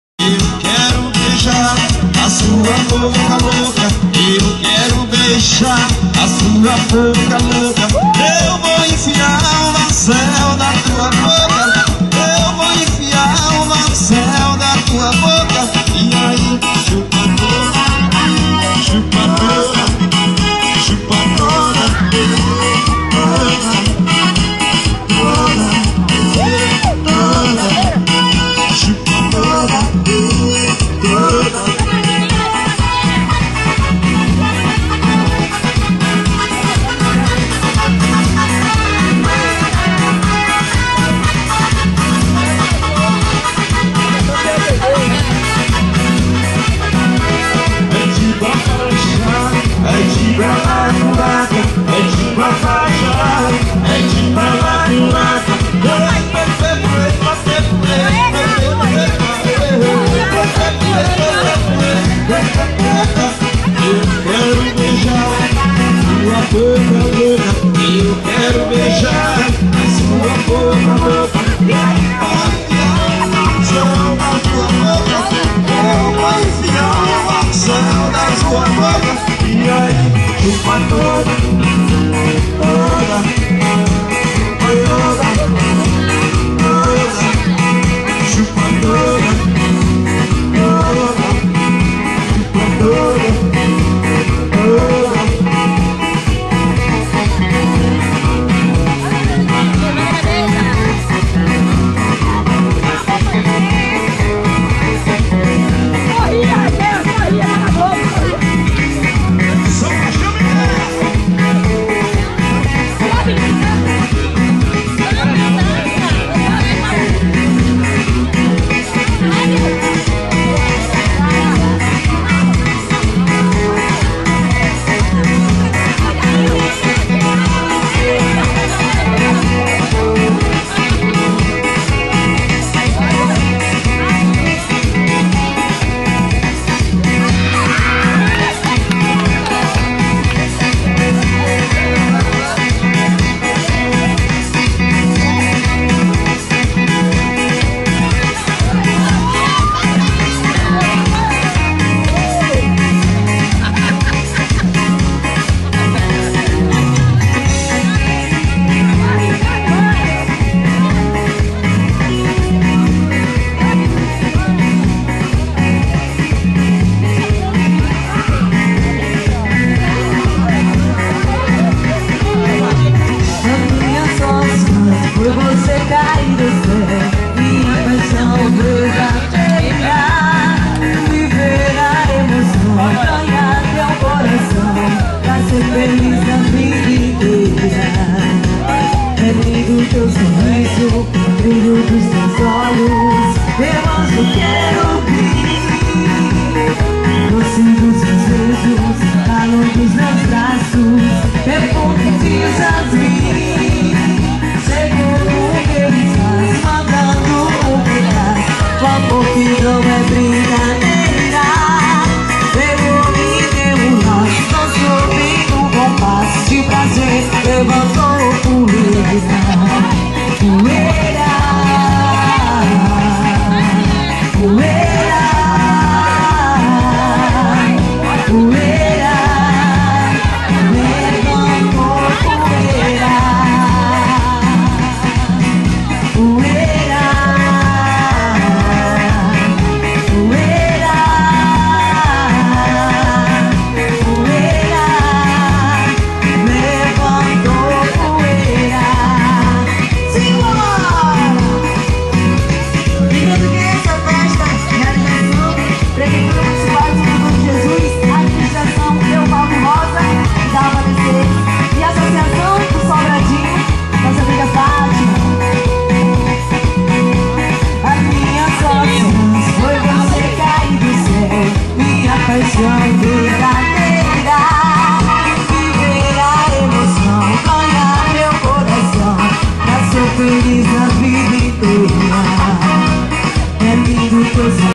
TONHA MAMUSKA - Santo Antonio de Jesus-Ba
Professora, divertida, humorada. Nesse vídeo, momento antes de conduzir uma quadrilha junina em julho.07 na comunidade de Sobradinho, S.A. de Jesus-BA. Fizemos uma simples ediçãom tornando-o mais divertido.